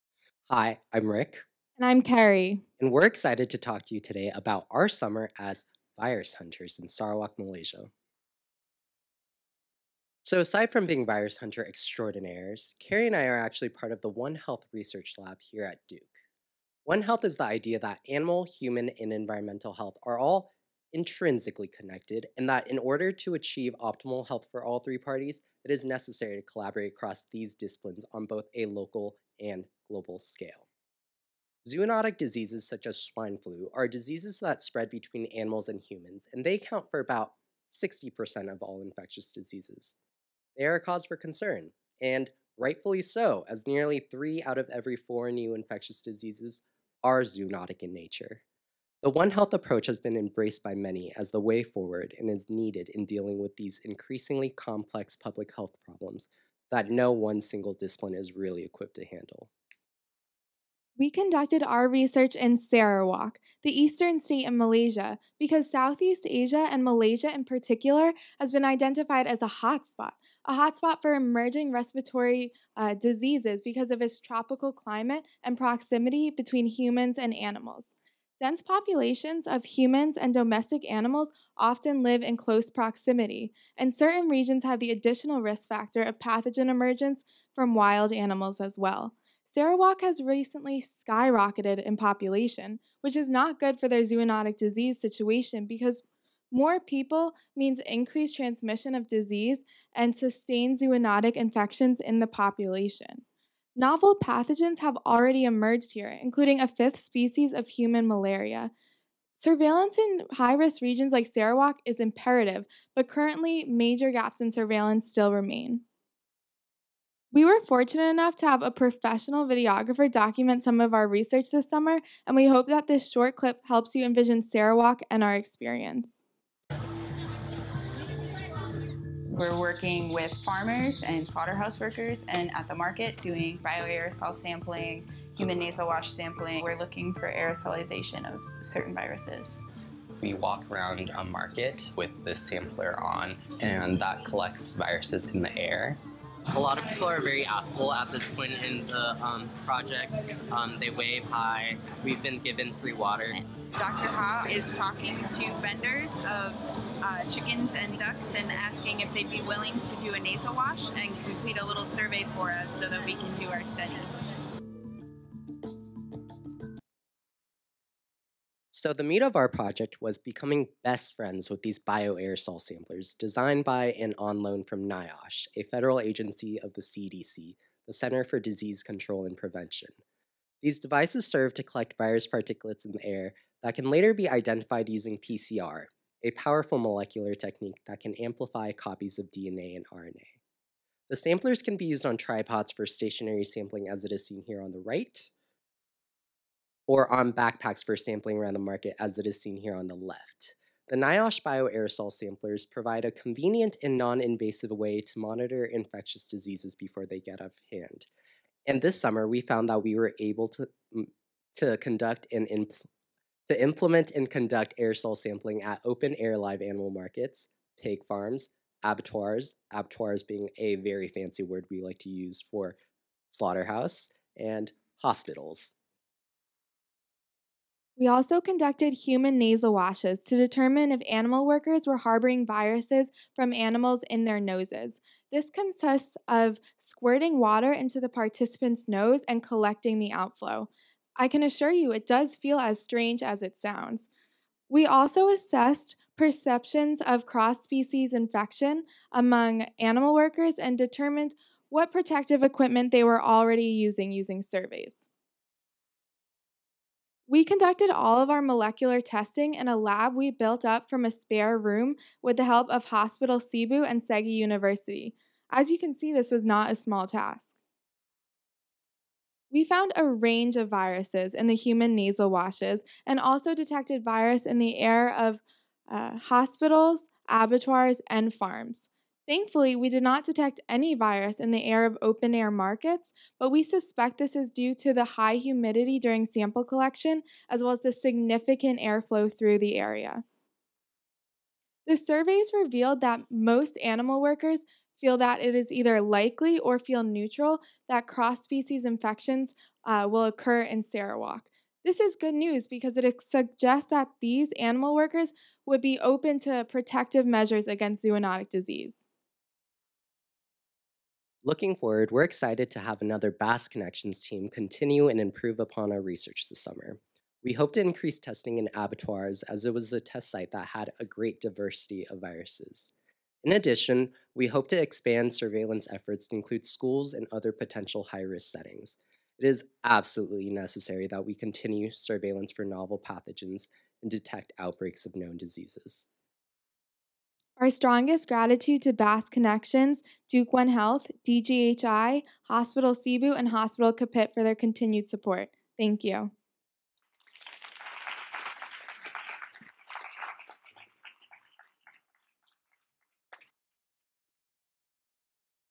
Building Capacity for Surveillance and Diagnosis of Respiratory Viruses in Sarawak, Malaysia
Duke University students Kerry Mallinson and Rick Tsao gave a talk about their Bass Connections project team at the Bass Connections Showcase on April 18, 2018.